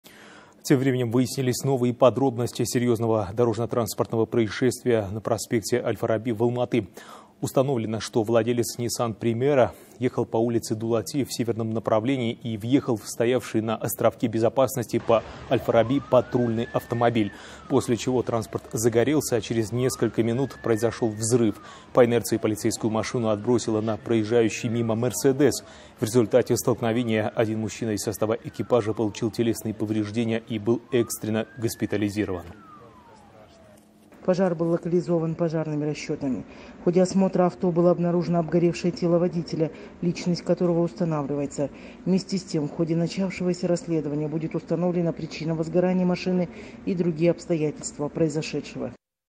Страшное ДТП в Алматы: автомобиль загорелся в центре города (28.08.20)
На проспекте Аль-Фараби в Алматы произошло ДТП. Владелец Nissan Primera ехал по улице Дулати в северном направлении и въехал в патрульный автомобиль, стоявший на островке безопасности по Аль-Фараби. После чего транспорт загорелся, а через несколько минут произошёл взрыв. По инерции полицейскую машину отбросило на проезжающий мимо Mercedes. В результате столкновения один мужчина из состава экипажа получил телесные повреждения. Его экстренно госпитализировали, передает Almaty.tv.

"Пожар локализован пожарными расчетами. В ходе осмотра авто обнаружено обгоревшее тело водителя, личность которого устанавливается. Вместе с тем, в ходе начавшегося от расследования будет установлена причина возгорания машины и другие обстоятельства произошедшего", - заявила официальный представитель городского ДП Салтанат Азирбек